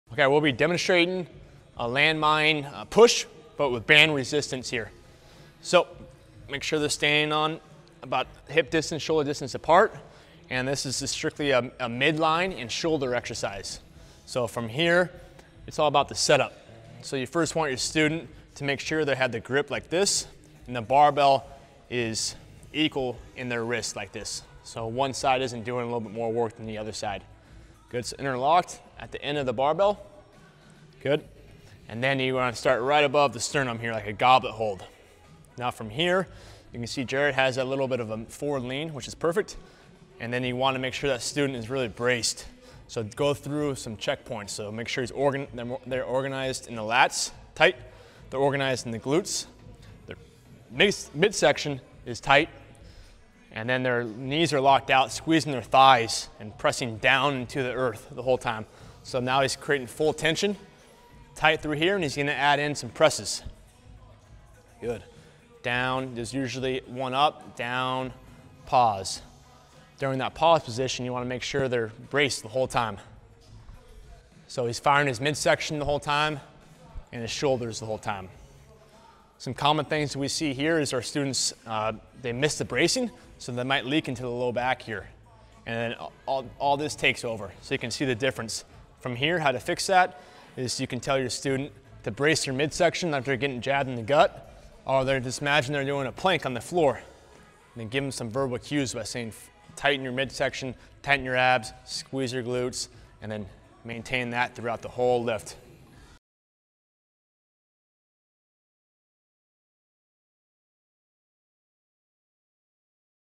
Landmine Push with Band Resistance Tutorial
Coach Jared and Coach Richie properly demonstrate how to do a landmine push with band resistance as well as some common mistakes.